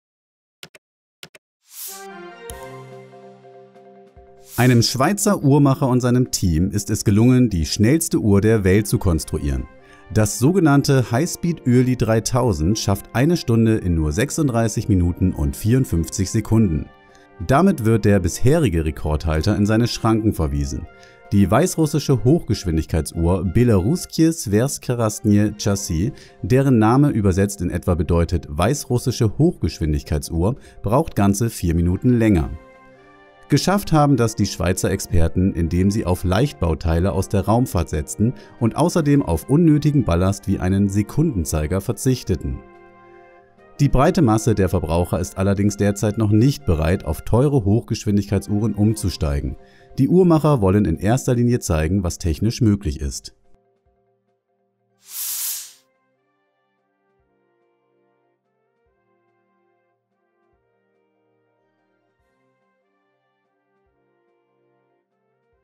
Neuer Rekord! Schnellste Uhr der Welt schafft Stunde in unter 37 Minuten
Uhrentuning extrem.